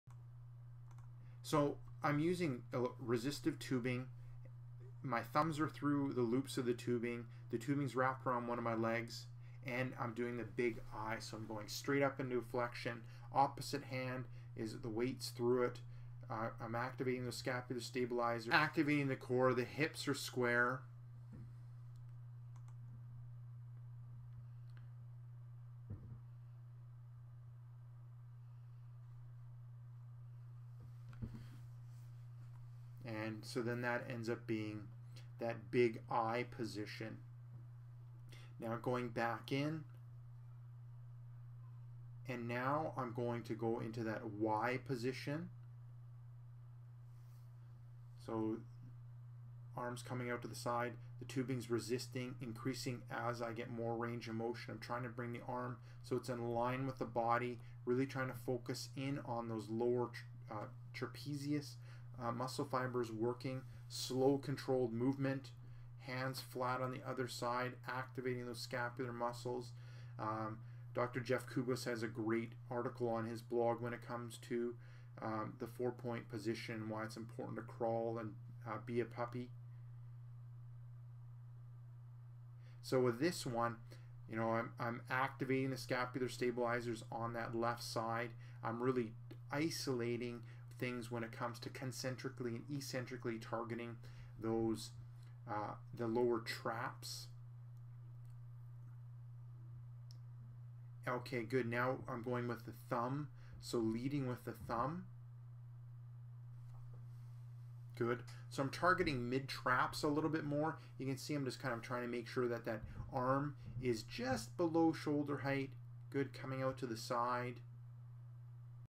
MIRU Scapular Stabilization Exercise
In this video, Rick Kaselj performs the scapular stabilization exercise using tubing.